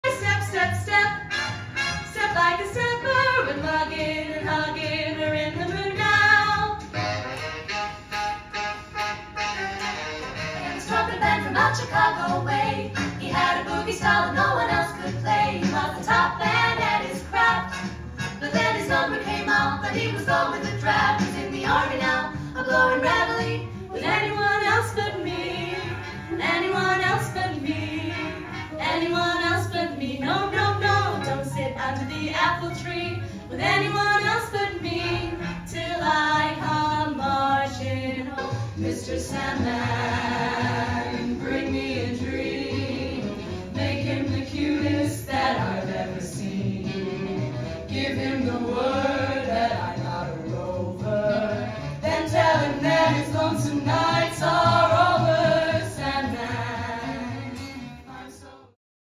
The D Day Dollies
A sneak clip from the D-Day Dollies 'Remembrance Week' tour 2023. Mini Concerts is a non for profit organisation that provides affordable high quality entertainment for those living in Care and Nursing Homes.